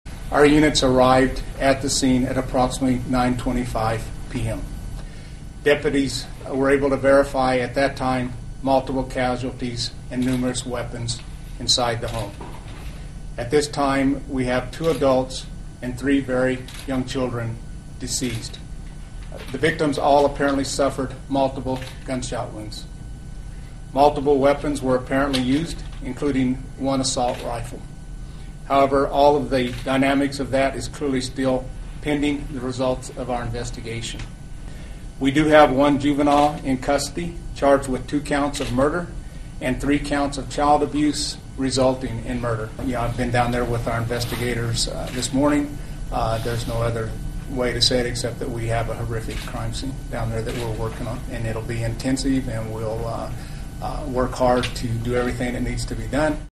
Sheriff: 'Horrific Crime Scene'
New Mexico Police say the 5 people found shot dead were a Fire Department chaplain, his wife and three of their children. The victims were shot multiple times with an assault rifle and other weapons. (Jan. 21)